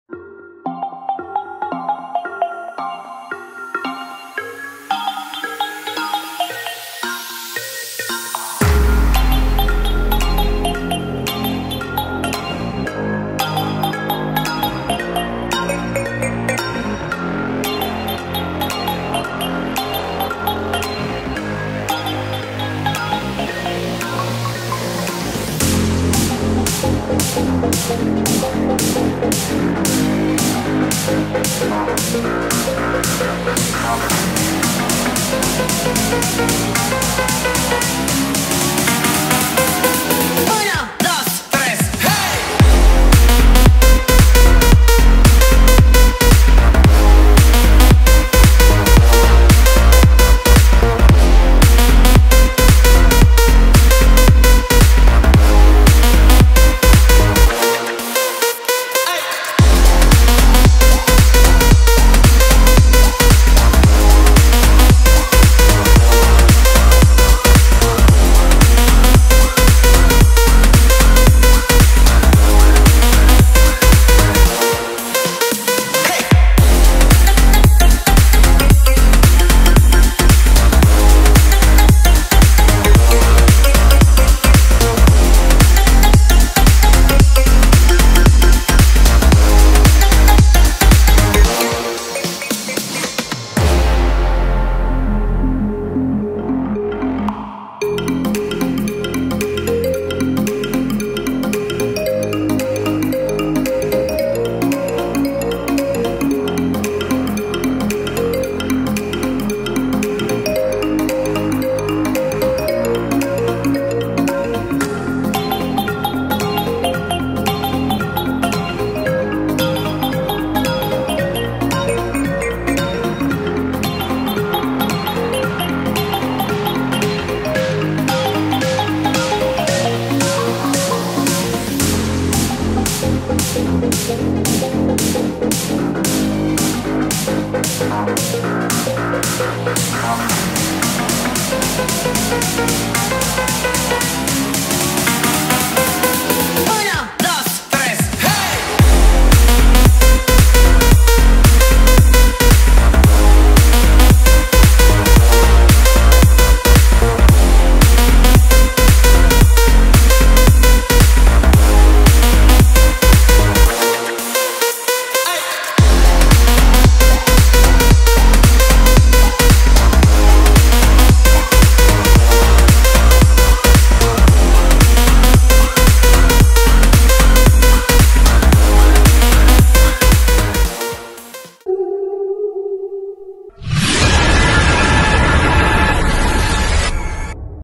Making ALL Garten of Banban chapter 2 / Cartoon VS Realistic
Hi guys! I'm glad to see you on my channel!  ❤❤❤
Today I'm making All Garten of Banban chapter 2 with clay, Cartoon VS Realistic.
Good luck to all! See you soon! 🖖